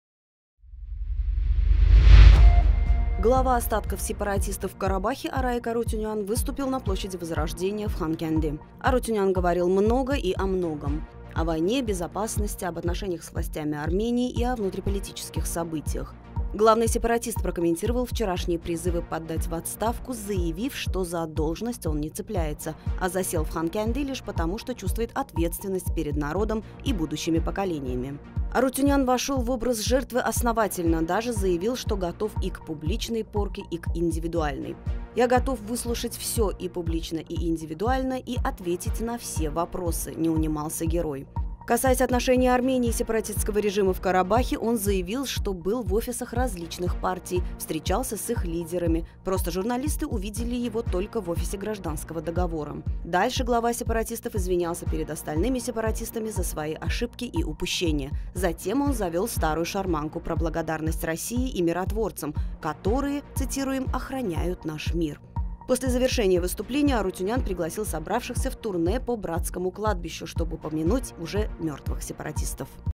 Не корысти ради… Араик Арутюнян: я не цепляюсь за должность, я чувствую ответственность
Глава остатков сепаратистов в Карабахе Араик Арутюнян выступил на площади Возрождения в Ханкенди.
Арутюнян говорил много и о многом. О войне, безопасности, об отношениях с властями Армении и о внутриполитических событиях.
Главный сепаратист прокомментировал вчерашние призывы подать в отставку, заявив, что за должность он не цепляется, а засел в Ханкенди лишь потому что чувствует ответственность перед народом и будущими поколениями.
Арутюнян вошел в образ жертвы основательно, даже заявил, что готов и к публичной порке, и к индивидуальной. «Я готов выслушать и публично, и индивидуально, я ответить на все вопросы», - не унимался герой.
Касаясь отношений Армении и сепаратистского режима в Карабахе, он заявил, что был в офисах различных партий, встречался с их лидерами, просто журналисты увидели его только в офисе “Гражданского договора”.
Дальше глава сепаратистов извинялся перед остальными сепаратистами за свои ошибки и упущения. Затем он завел старую шарманку про благодарность России и миротворцам, которые, цитируем, охраняют наш мир.
После завершения выступления Арутюнян пригласил собравшихся в турне по Братскому кладбищу, чтобы помянуть уже мертвых сепаратистов.